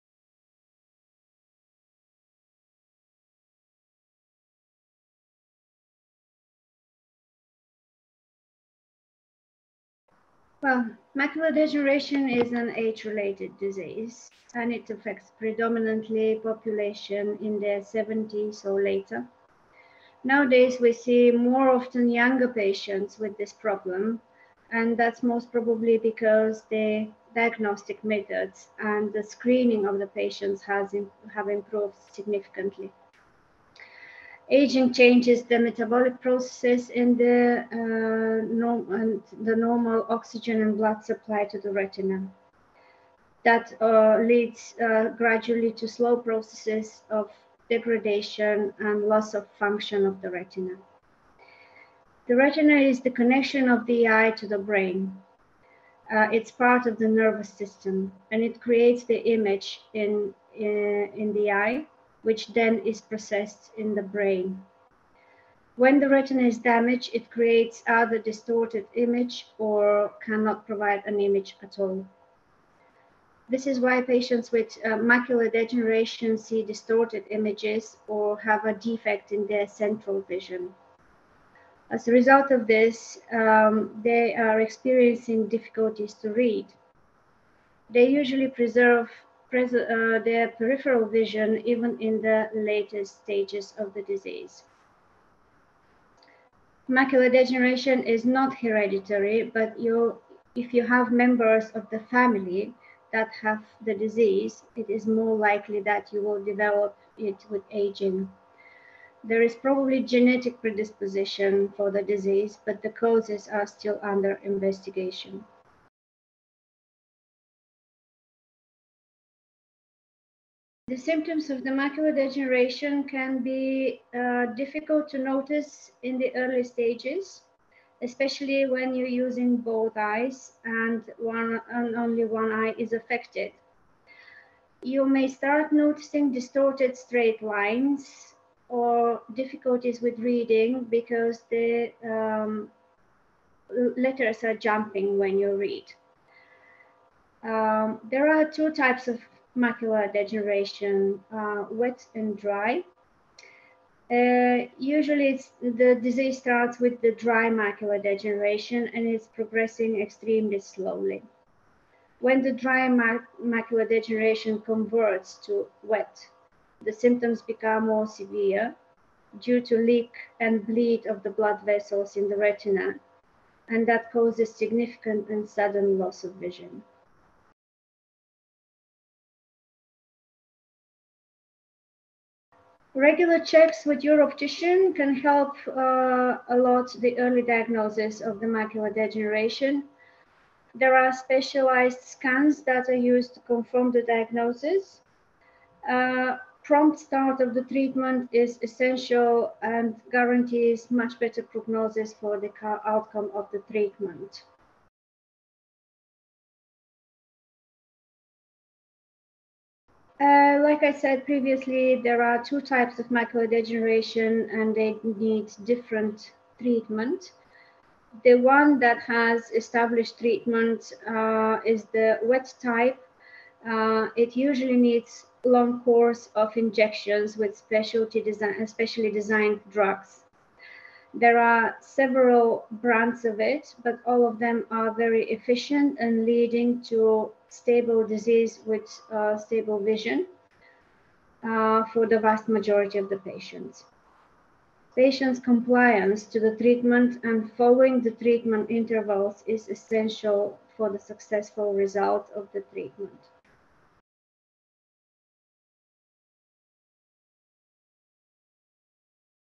What exactly is macular degeneration? - Online interview
Macular degeneration is a condition that affects the central vision as people age, usually symptoms appear when a person is in their 50s. Revered consultant ophthalmologist Mrs Kapka Nenova, based in East Grinstead, explains the disease in further detail. She explains the causes and highlights the symptoms to be aware of, before outlining the diagnosis and treatment.

00:00 What is macular degeneration?
01:56 What are the symptoms?
02:58 How is macular degeneration diagnosed?
03:25 How is macular degeneration treated?